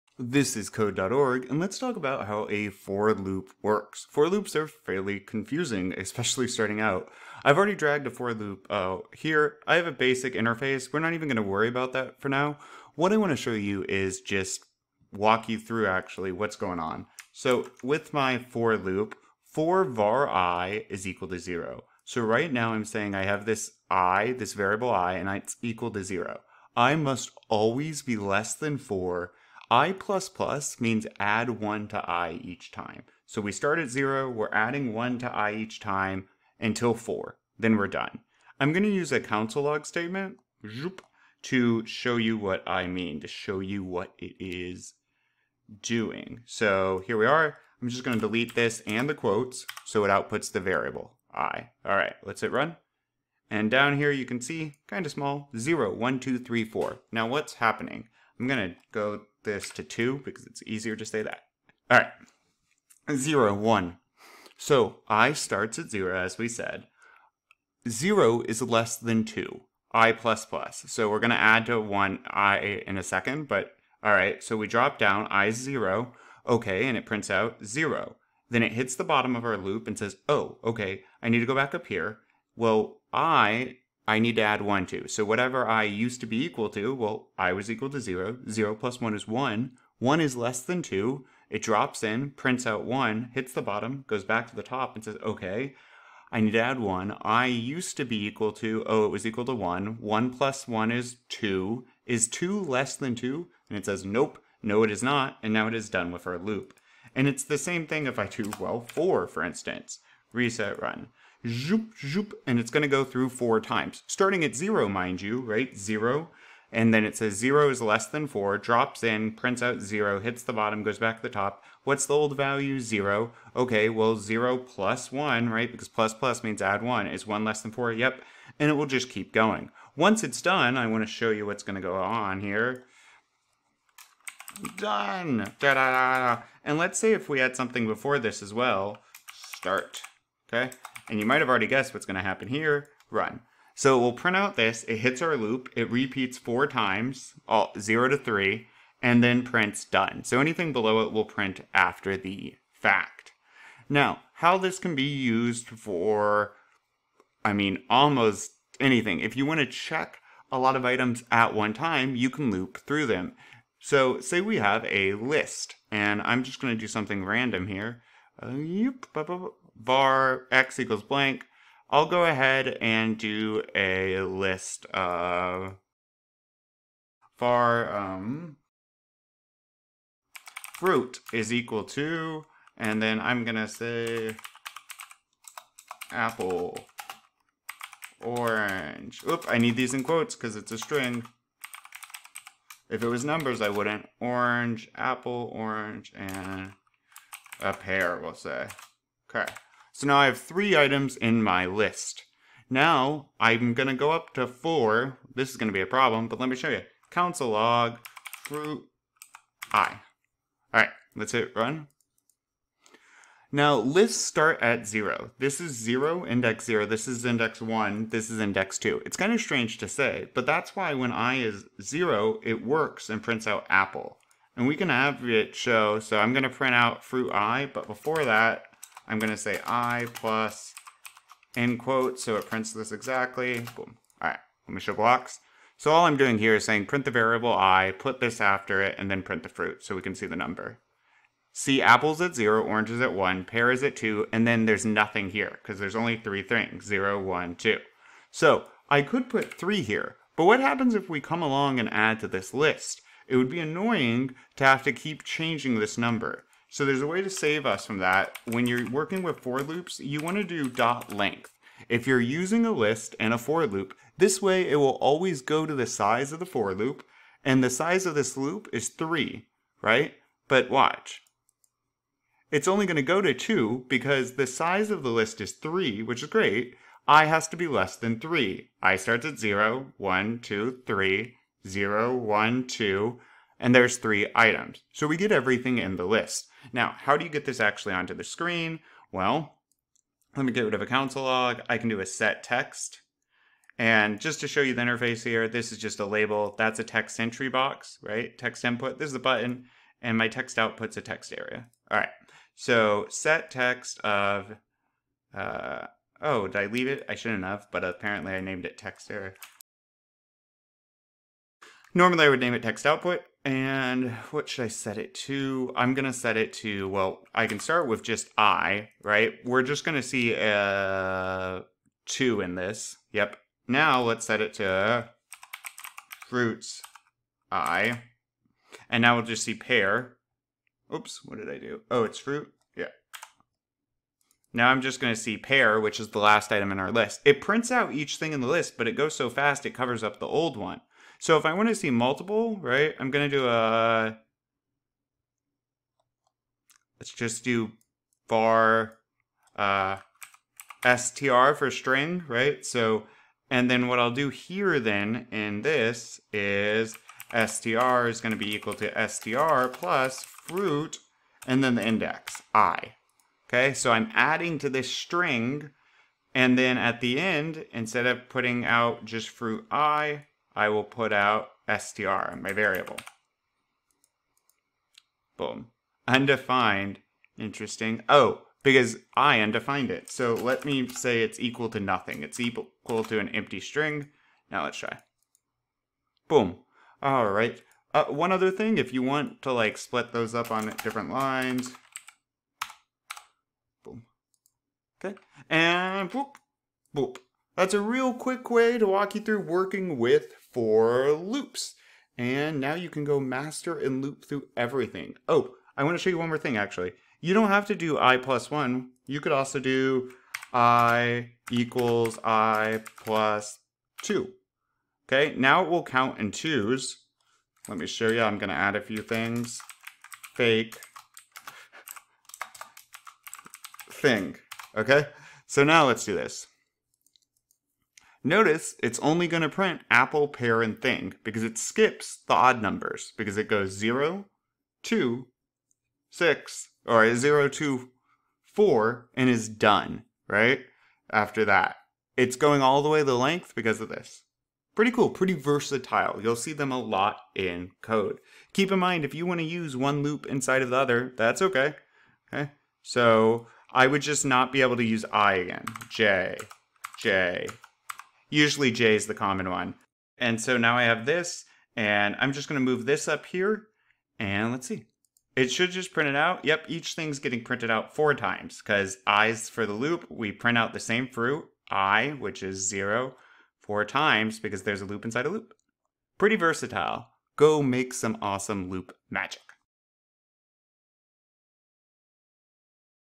Code.org For Loop Tutorial - How Do Loops Work? Hackathon | Unit 6 CSP
Hackathon Time! Learn tips and tricks to create an awesome app! Oh, Lists, Loops, and Traversals is unit 6 of Code.org's C.S. Principles course. The course is often used in AP Computer Science classrooms. Each part of the course challenges students to accomplish a new task while learning the principles of computer science. In this video I walk through the step-by-step how to get the right answer for this puzzle.